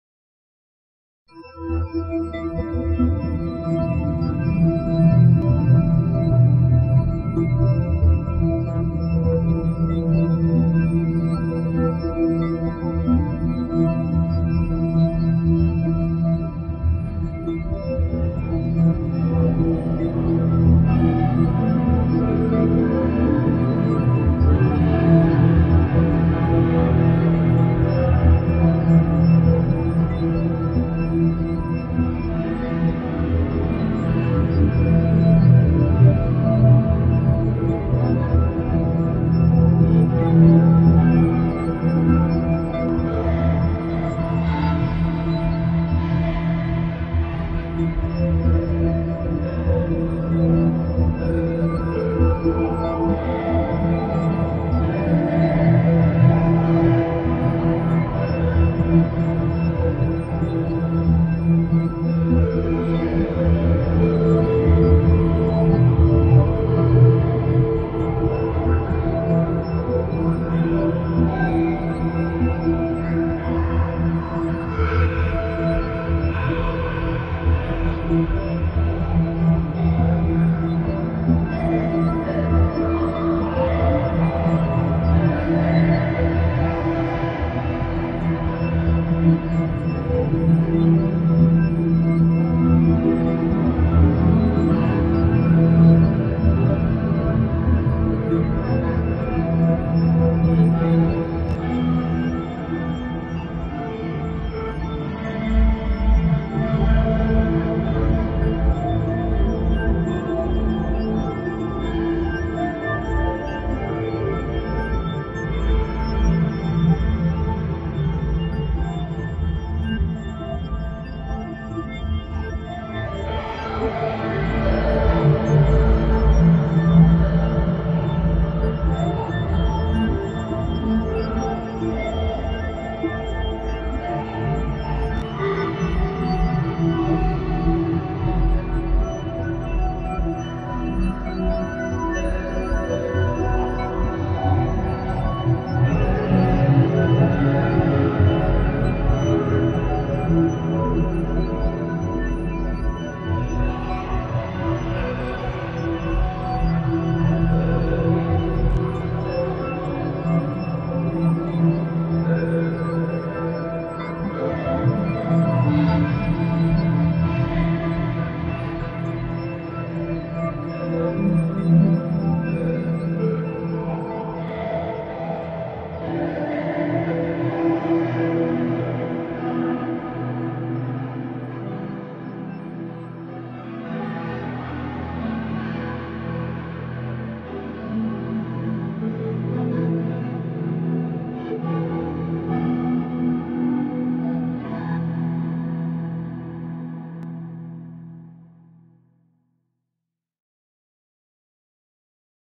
Kaleidoscope Subscription – Flying Flutes
Improvising with two Kaleidoscope presets from the patchpool subscription series - set no. 19 - changing root notes in the two instances on the fly.